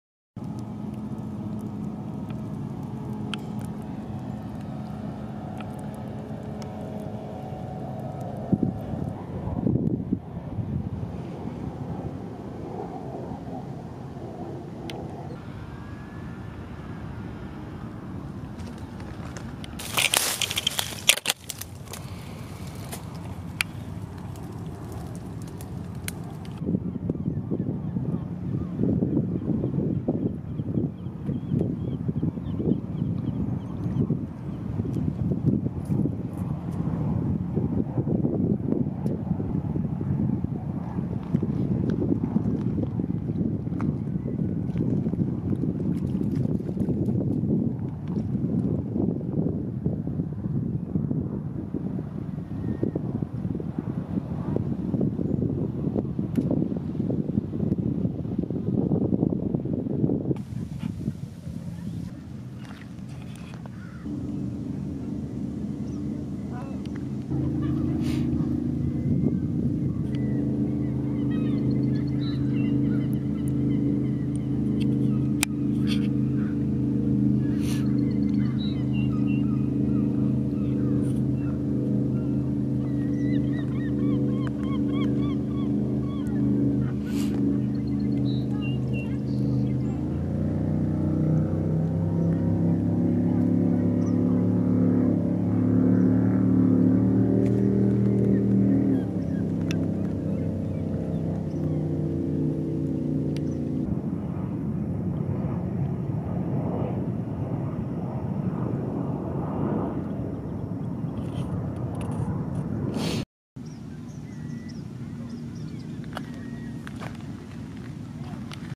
Nature Walk Ep.1
Episode one of maybe many. The crab featured in the video was dropped by the seagull shown immediately before it, the one that was just soaring around.